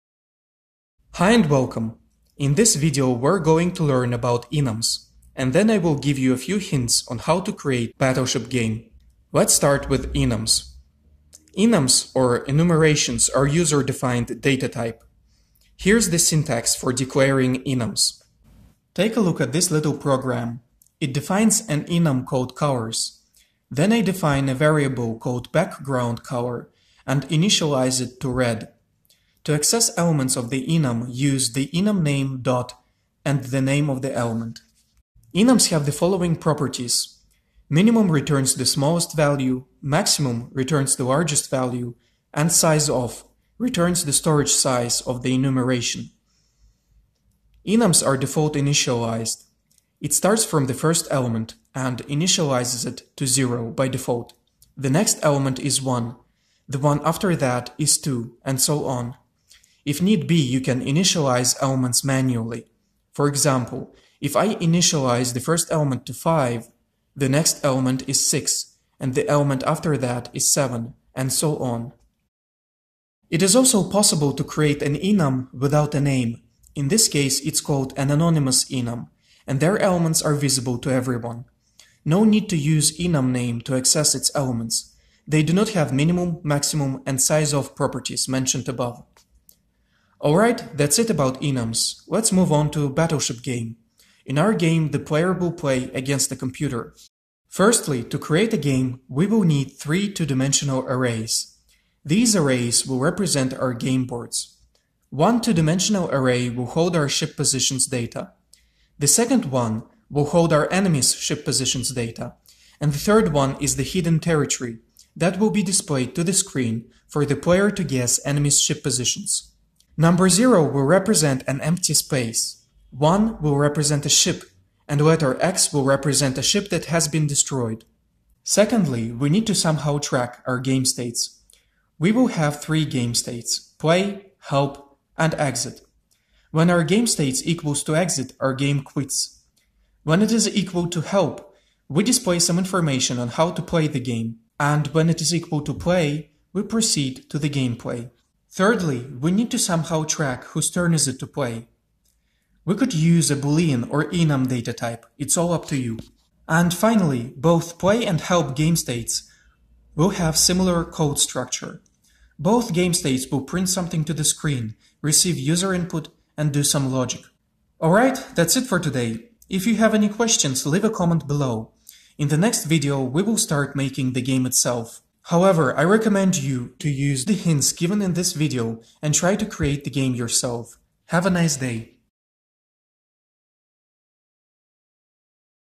#9.1 | Enums | How to create Battleship game (hints) | Let's learn Dlang game dev
Hi!

In this video we learn how to use enums, and I give a few hints on how to create Battleship game.